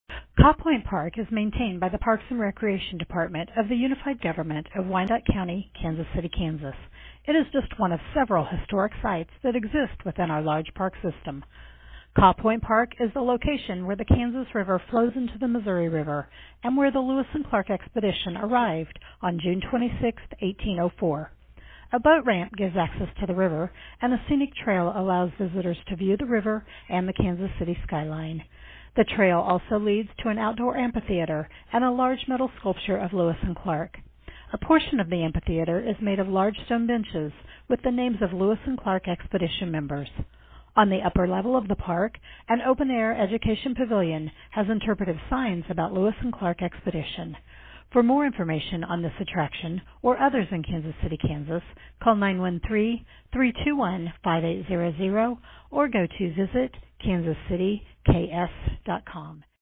Kaw Point Park Virtual Tour
Learn a little about the historic Kaw Point Park! This riverside park is known to be a spot where the Lewis and Clark expedition party camped from June 26–28, 1804, on their way from St. Louis to the Pacific Ocean.

Today visitors can bike, fish and explore the area, all with an iconic view of the downtown Kansas City skyline. Sound on for your very own virtual tour of this historic site in KansasCityKS. 🔊